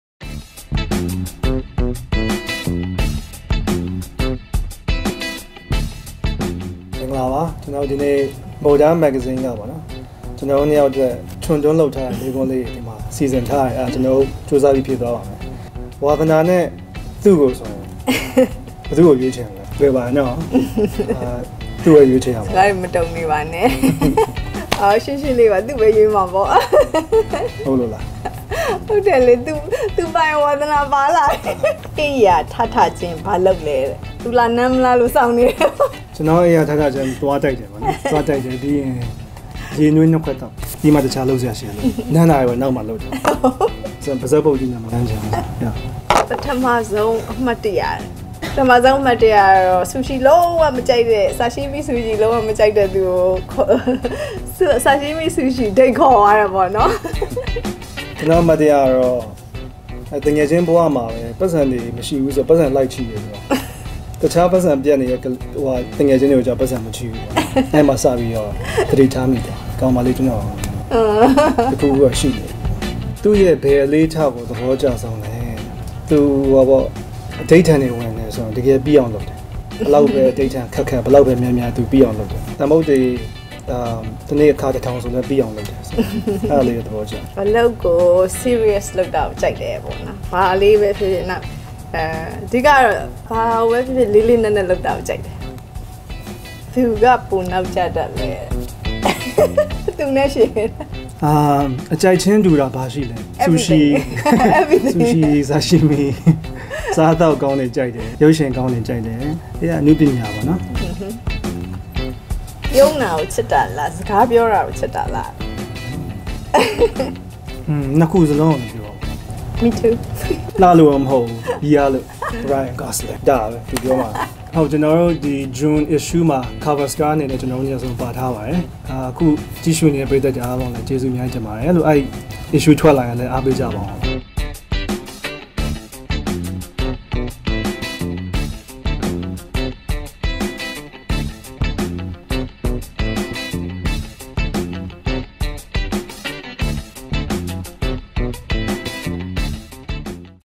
ZENN KYI | CHRISTINA KYI | MODA ASKS | PART 1 | MODA FASHION MAGAZINE | MODA MYANMAR
Christina Kyi ကို ဘာေၾကာင့္စိတ္၀င္စားသြားလဲဆိုတဲ့ အေၾကာင္း ေျပာျပသြးတဲ့ Zenn Kyi